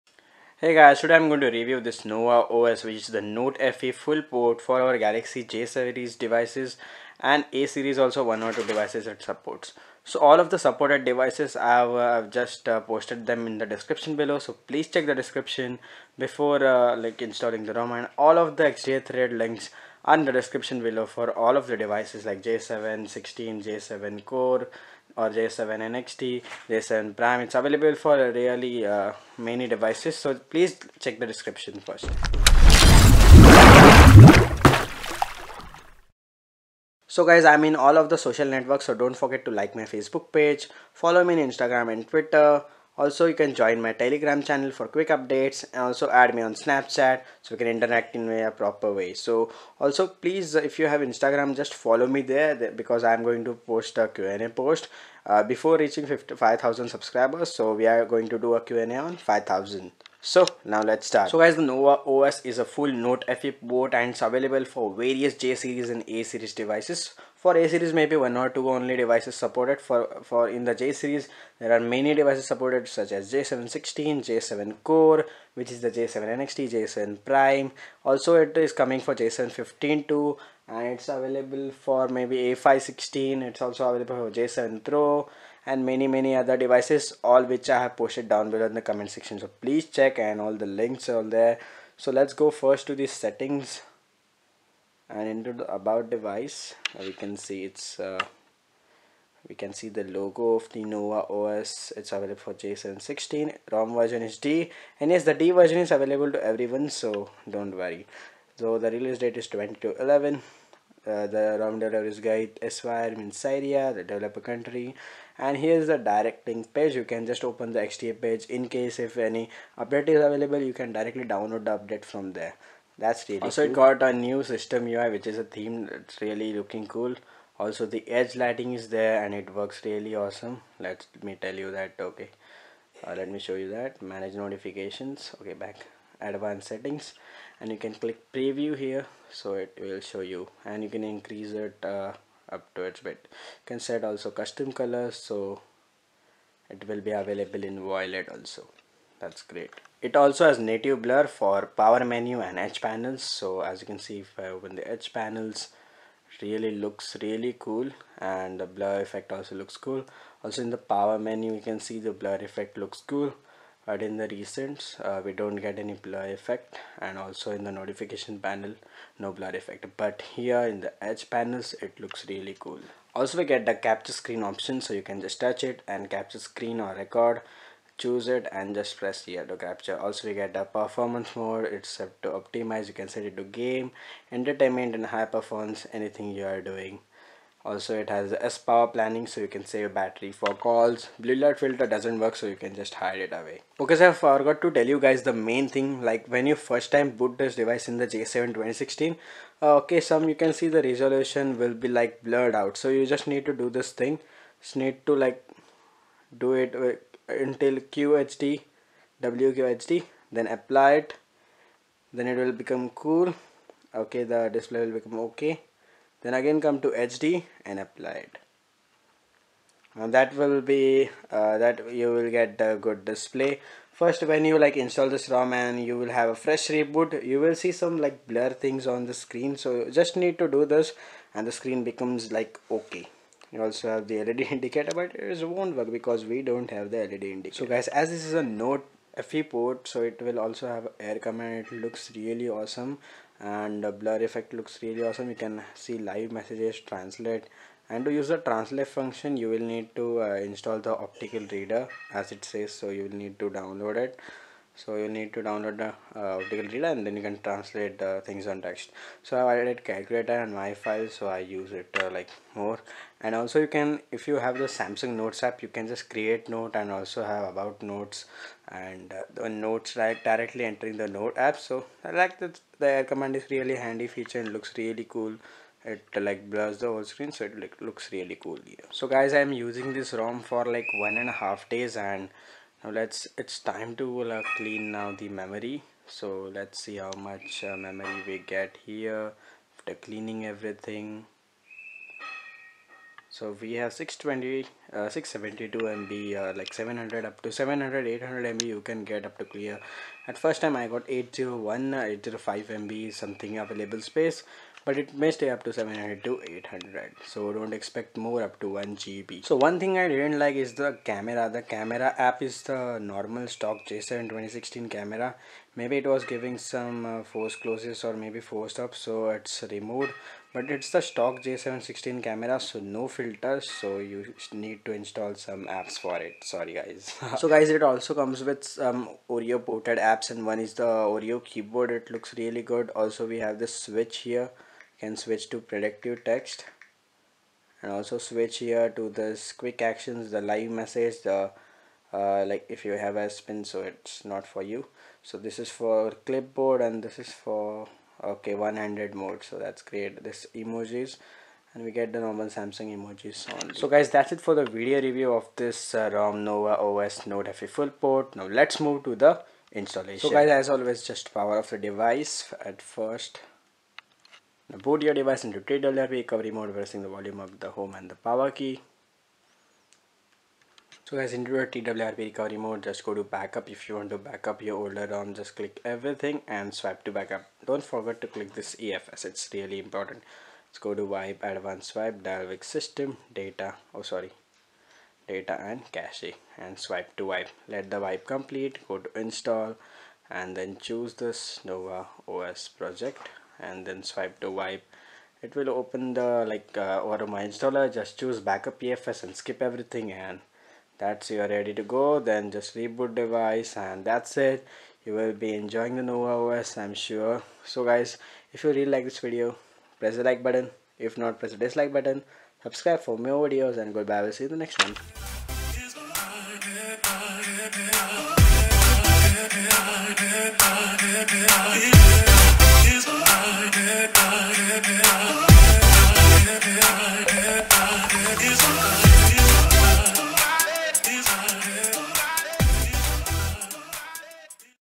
Nova OS [Version D] For Galaxy J Series & A Series
In this video, I've reviewed the Nova OS which is a Note FE Full Port with features like Edge Lightning and Air Command.

It is available for various devices.

Supported Devices -
J7 Prime
J7 Pro
J7 Nxt
J7 2015
J7 2016
A3 2017
A5 2017
J5 2017
J7 2017

Status - Stable - Suitable for daily usage.

Credits - GhaithSYR (Senior Member, XDA)

Features -
Air Command
Edge Panels
Edge Lightning
Native Blur
Capture & Record Screen Toggles
Video Enhancer
Caller ID & Spam Protection
Note8 System UI Themed

Bugs -
Nothing :)